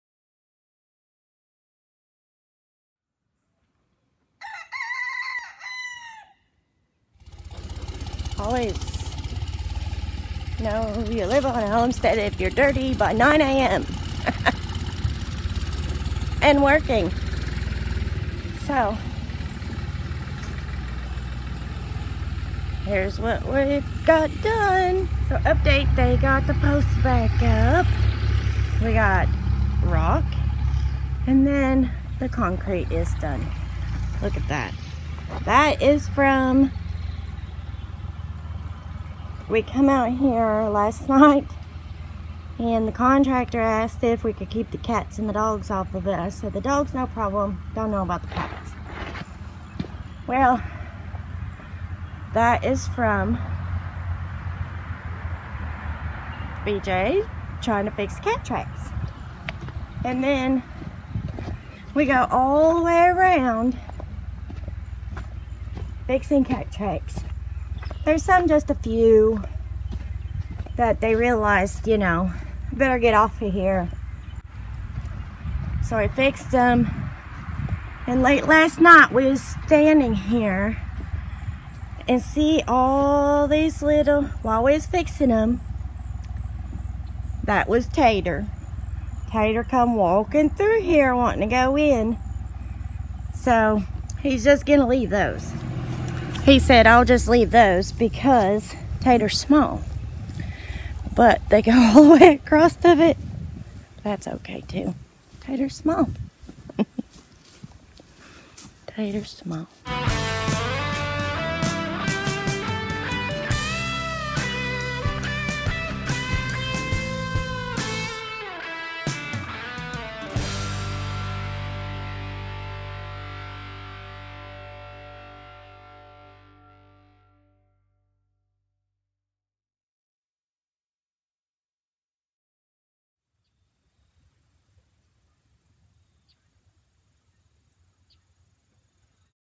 Concrete and animals
Cats and new concrete. Update on garage build and plenty more work to do!